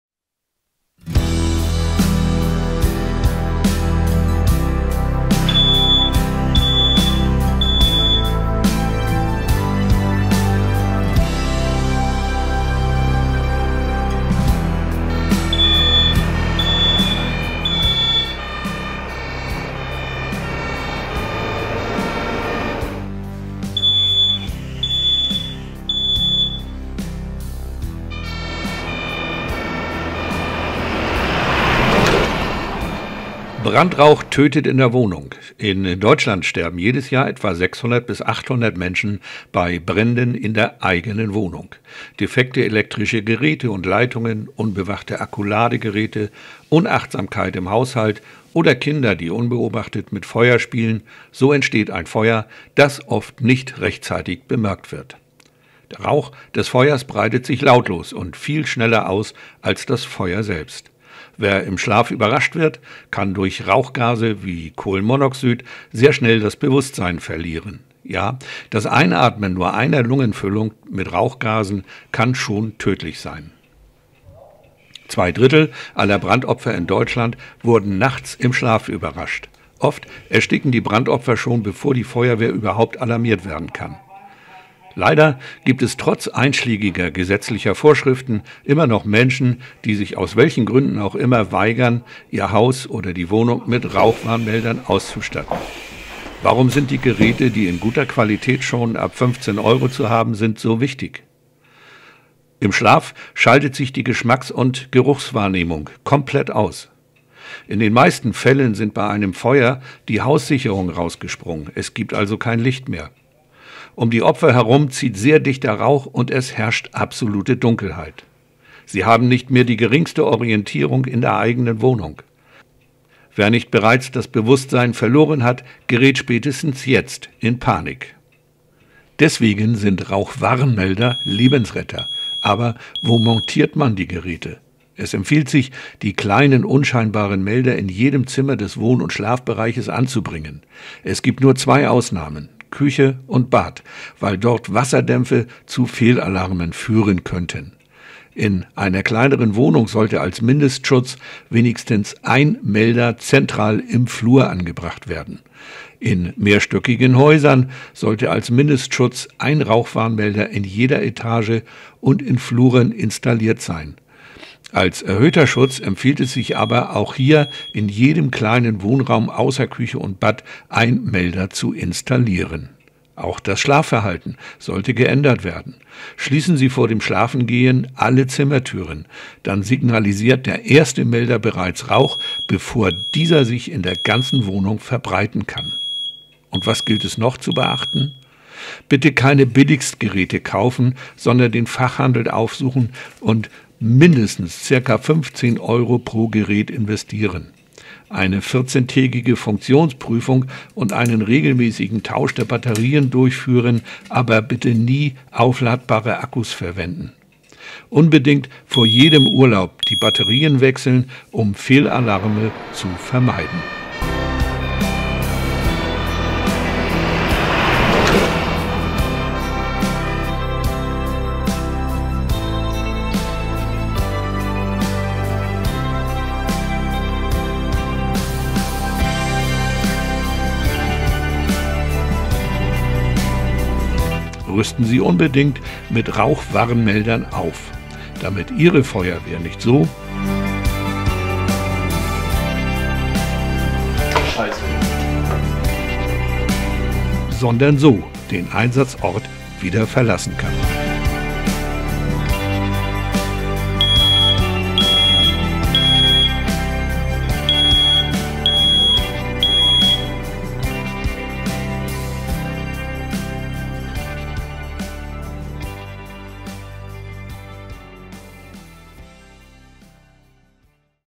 Brandrauch tötet - Rauch-Warnmelder retten Leben!!!
In Zusammenarbeit mit der Feuerwehr Molfsee ist ein Video über die Unverzichtbarkeit von Rauch-Warnmeldern entstanden. Jährlich 600 bis 800 tödlich endende Wohnungsbrände in Deutschland sprechen eine erschütternde Sprache: Rauchmelder retten Leben!